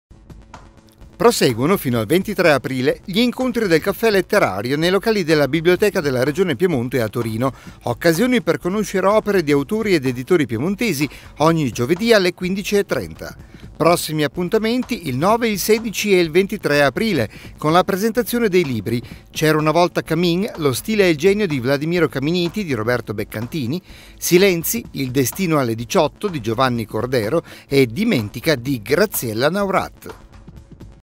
Caffè letterario
Proseguono fino al 23 aprile gli incontri del Caffè letterario nei locali della Biblioteca della Regione Piemonte a Torino, occasioni per conoscere opere di autori ed editori piemontesi, ogni giovedì alle 15.30. Prossimi appuntamenti, il 9, il 16 e il 23 aprile, con la presentazione dei libri  “C’era una volta CAMIN. Lo stile e il genio di Vladimiro Caminit” di Roberto Beccantini; “Silenzi. Il destino alle diciotto” di Giovanni Cordero e “Dimentica” di Graziella Naurath.